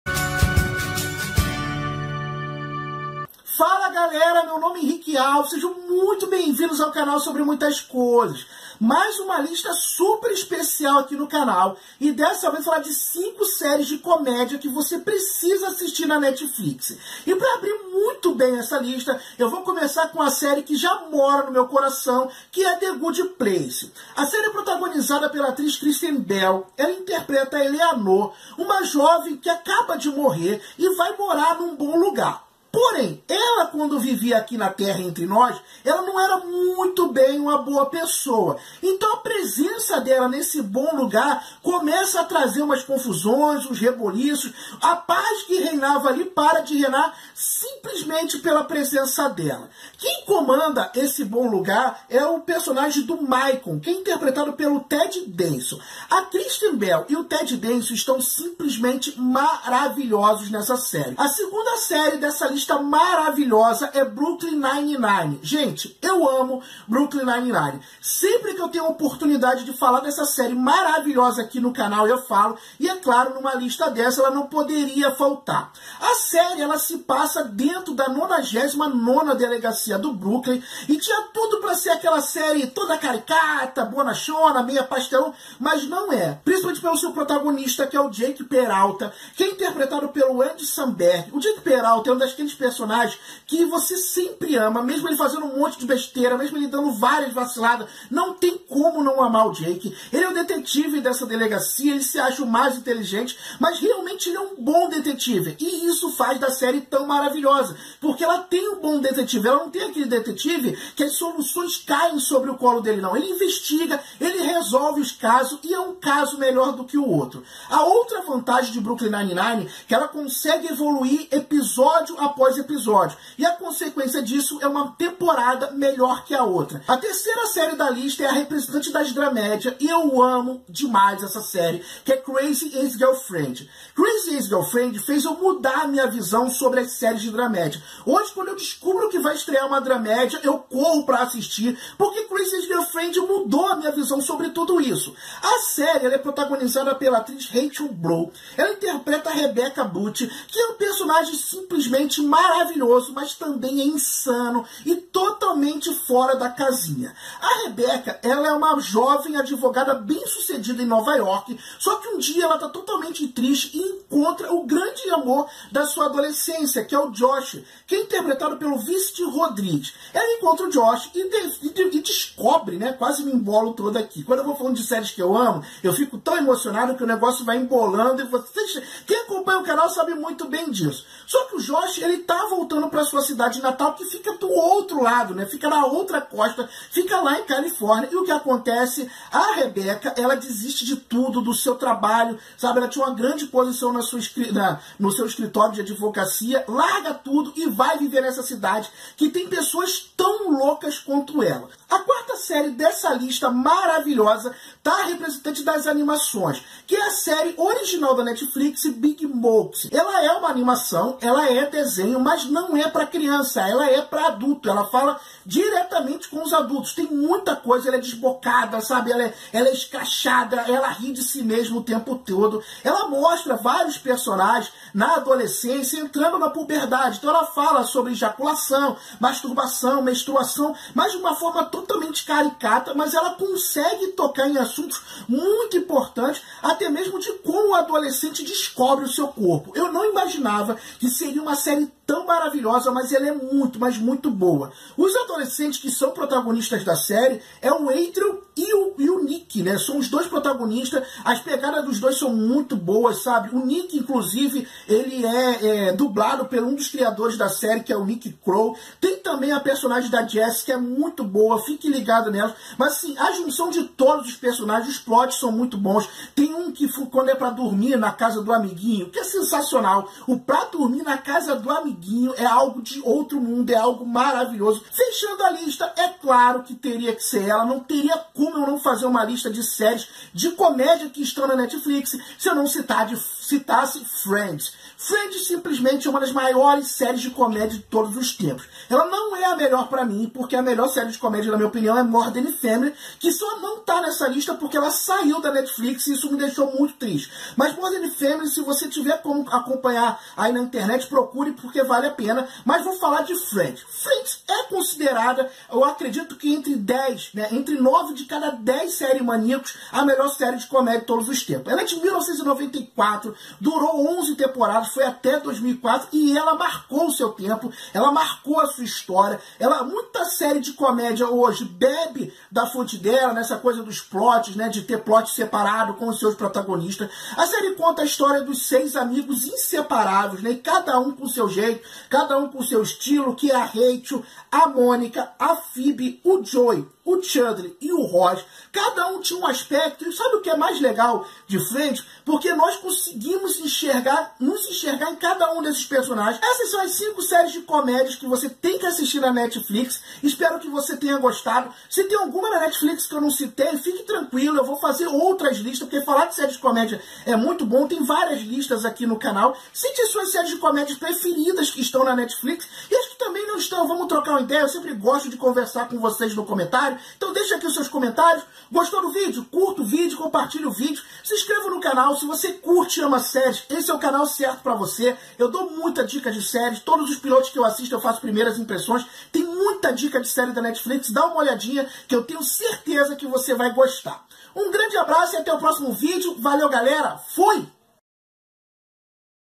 5 SÉRIES DE COMÉDIA PARA ASSISTIR NA NETFLIX
Estreia de quadro novo no canal, agora vai ter lista de séries de comédia para assistir na Netflix.

Lista de Séries Netflix
Lista de Séries de Comédia Netflix